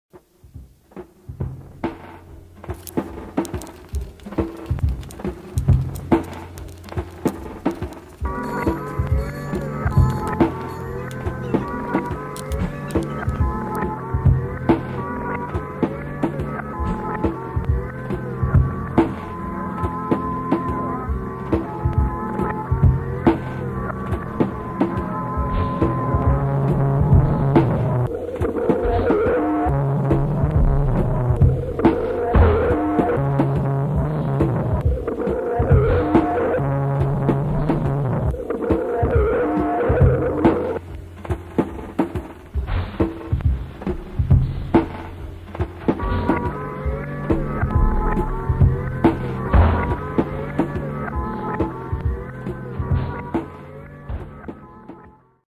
rapid abstract sketch assault
It does what it says on the tin: a rapid abstract sketch assault of wolverine crossed with a digestive biscuit